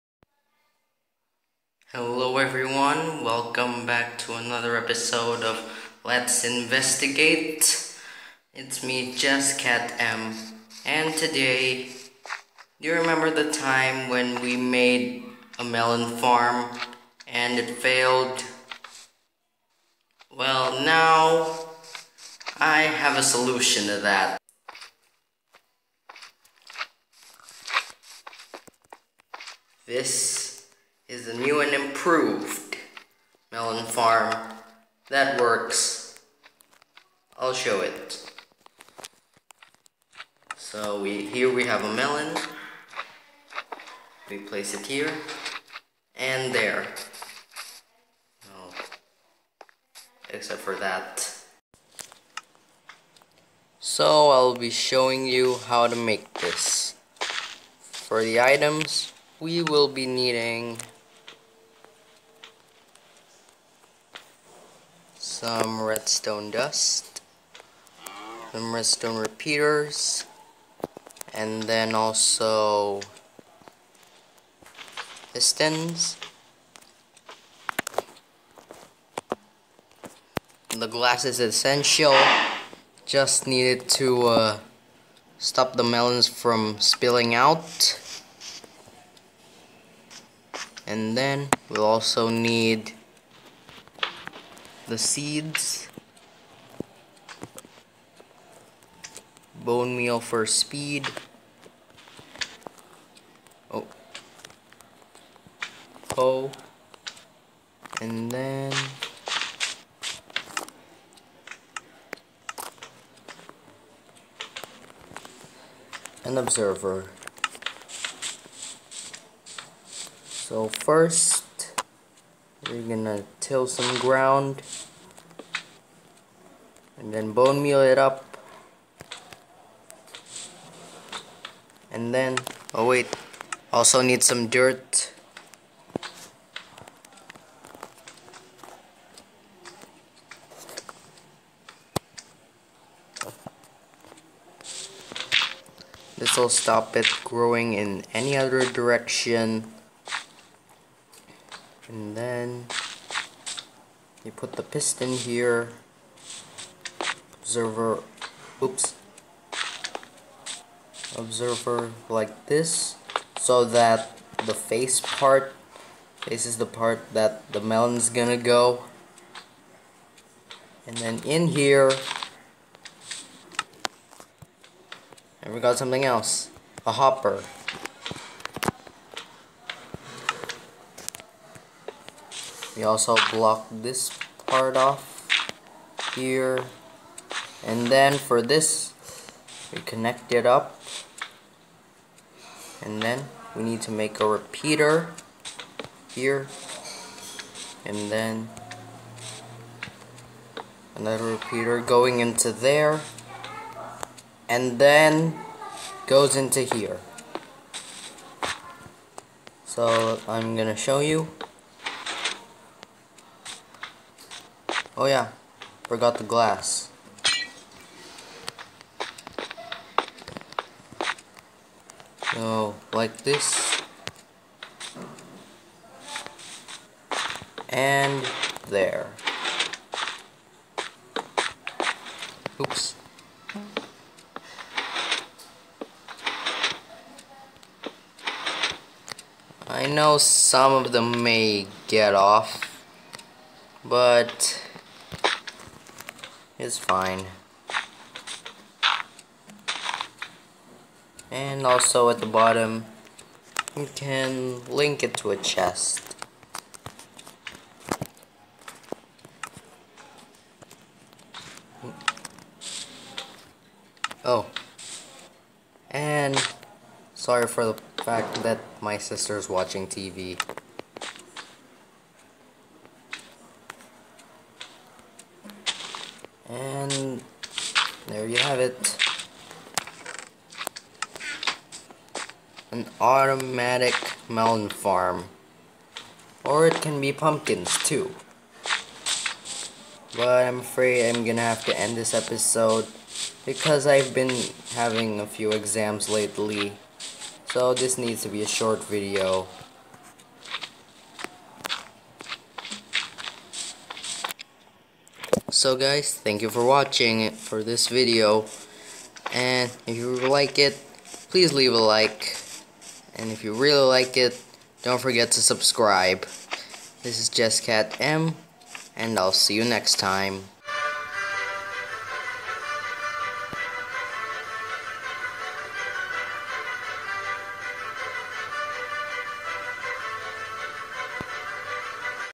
Let's Investigate Episode 3: WORKING Melon Farm!
In today's episode, we recreate the melon/pumpkin farm from last episode.

Our Instagram is @marcelinkatili.

So I hope you all enjoy it!